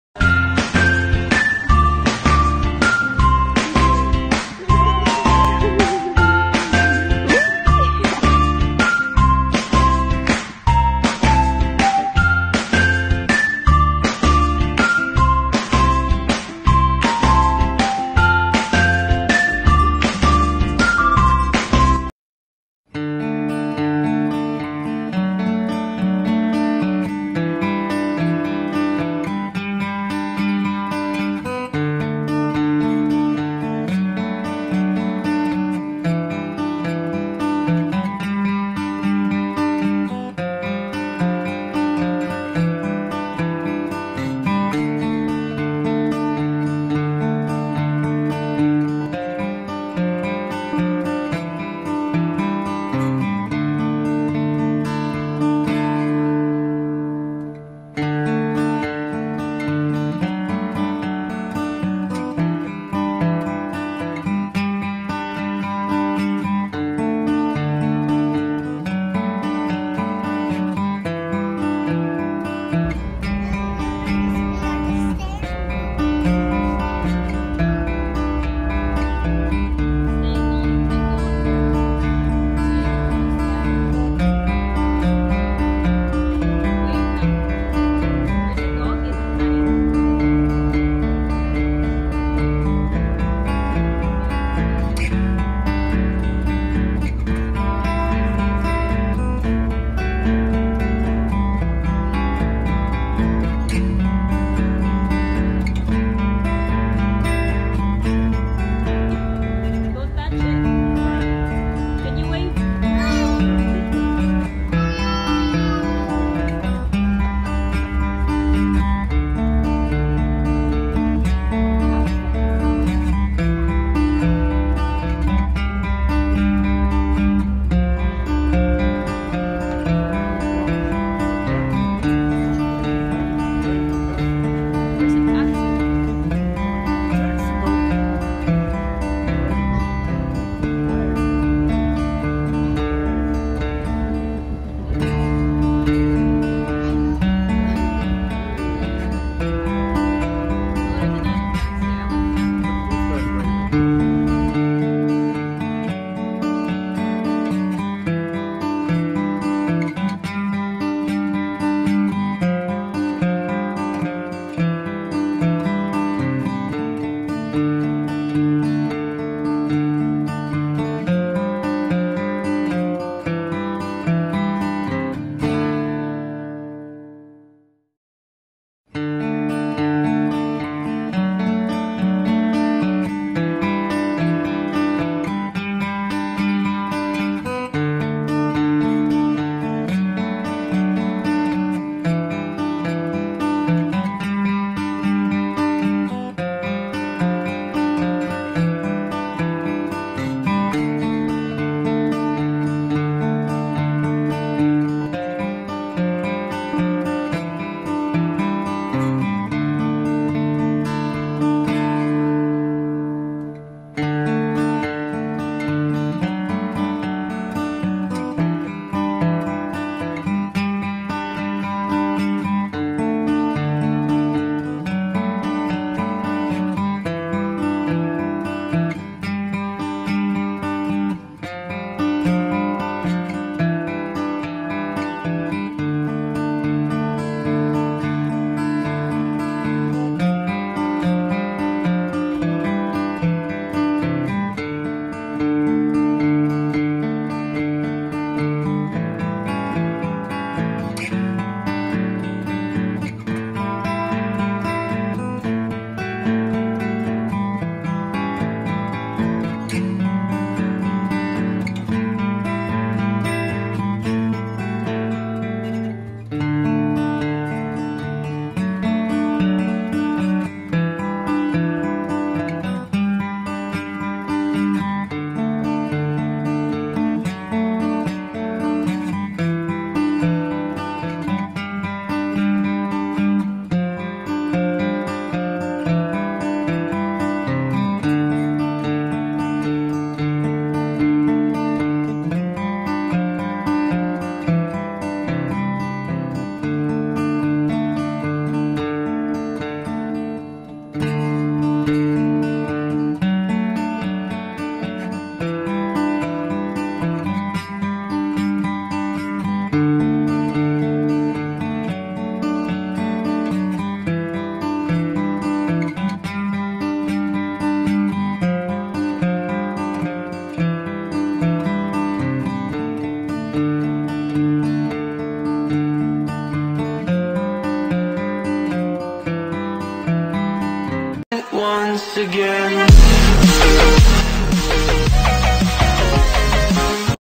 Traveling from Victoria CANADA to Washington USA by MV COHO || Toddler Traveler
Hello guys thank you so much for reading the description anyway this video is about Traveling to USA from Canada by Ferry and I’m sharing you guys what documents you need to bring when your traveling different country by Ferry . Please watch our video for more details and I thank you